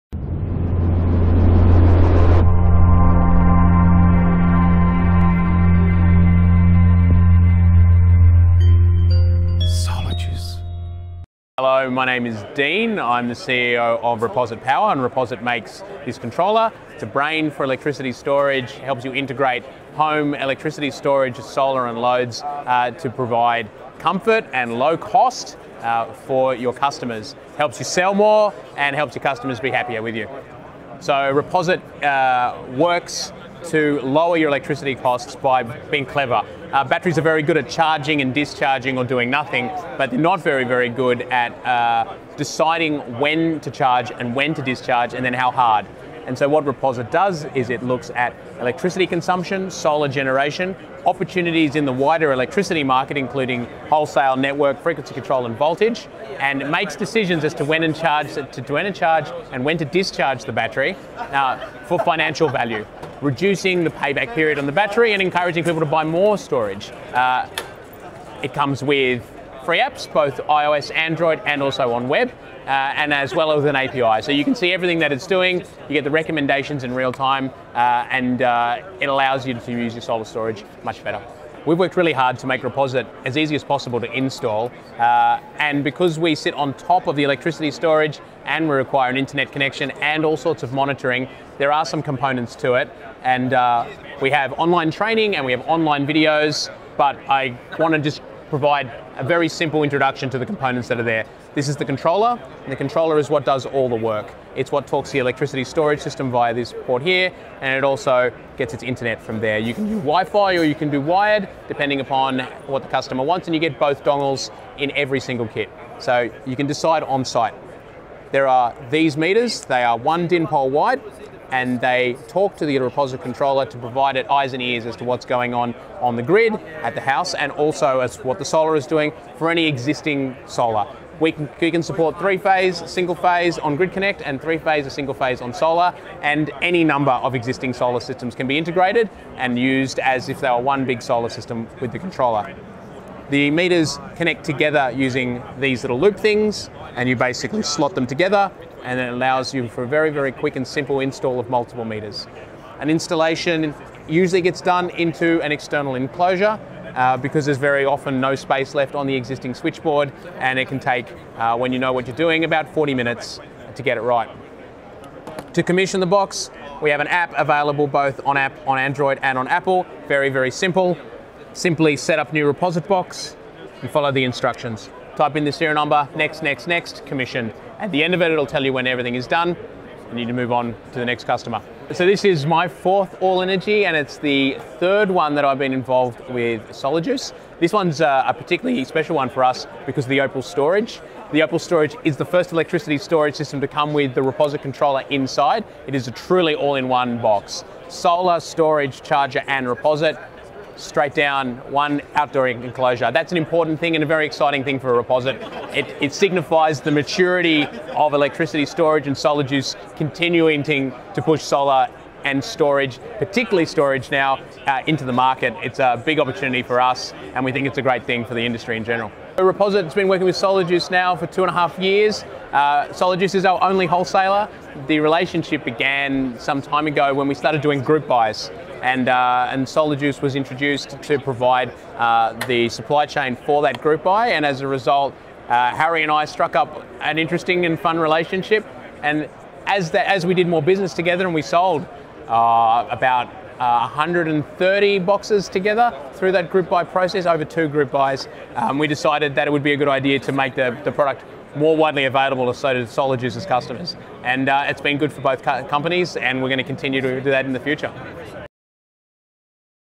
Reposit Power
Dean Spaccavento from Reposit Power gives us an explanation on how Reposit works, its components and the relationship with Solar Juice and Opal Storage.